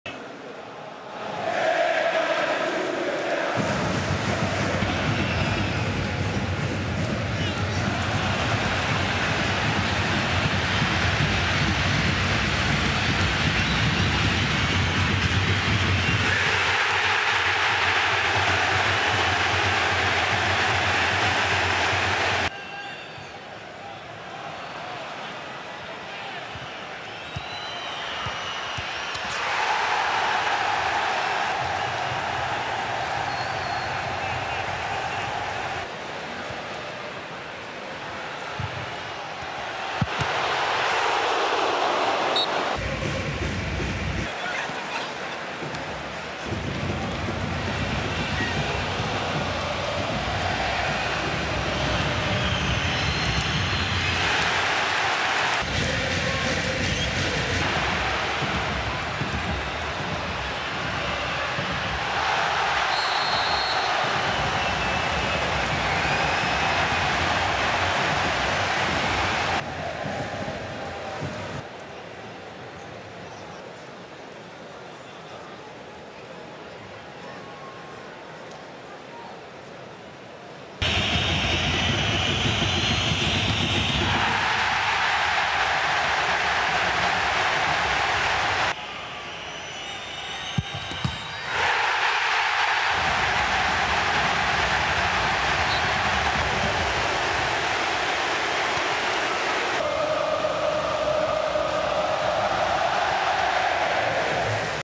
SAMPDORIA 1 (5) - (4) 1 COMO | Coppa Italia | Highlights
Como 1907 took on Sampdoria for its first official game of the season. The game ended 5-4 for Sampdoria on penalties.